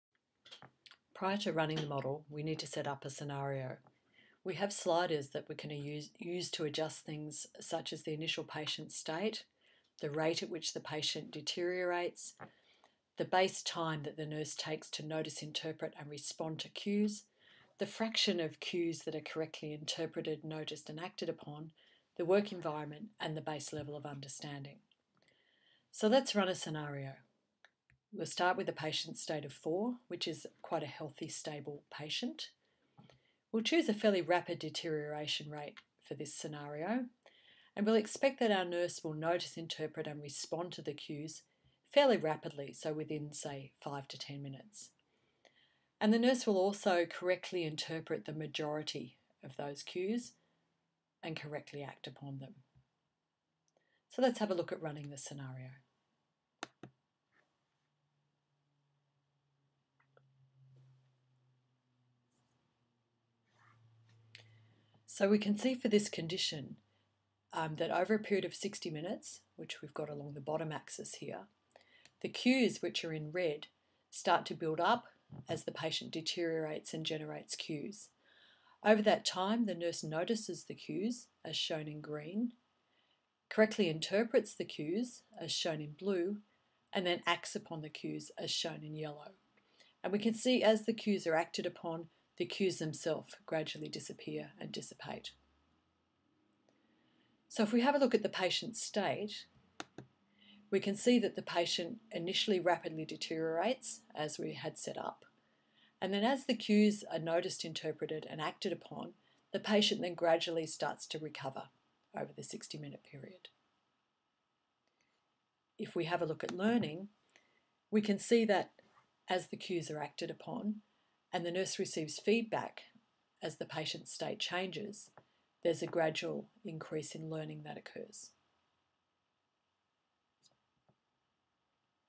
Thinking Like a Nurse - short.m4v
System dynamics model of Tanner's "Thinking Like a Nurse" - short version.